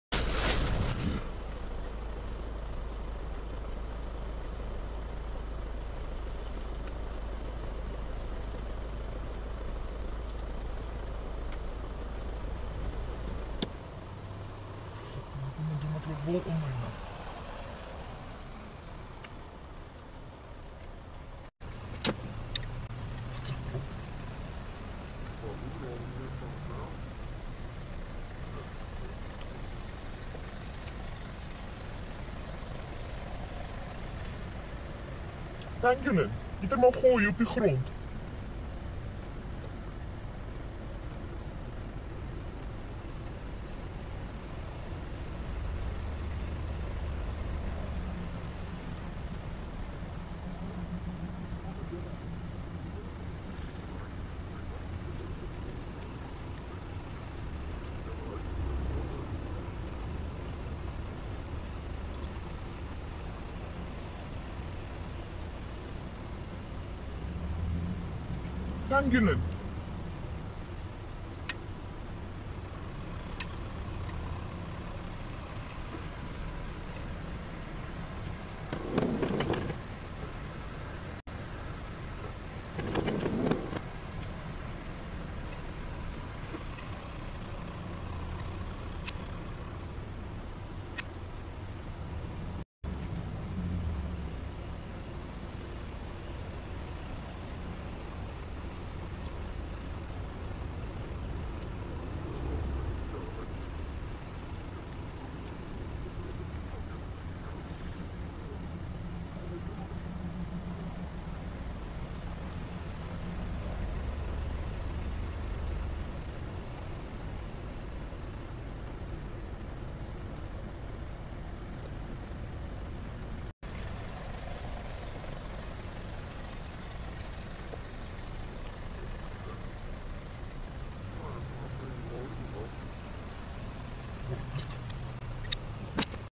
pangolin looking for food
Pangolins (or scaly anteaters) are mammals which live in Africa and Asia. They have scales on their skin. They are the only mammals which have this adaptation. There are three genera of pangolin, all of which eat ants and termites.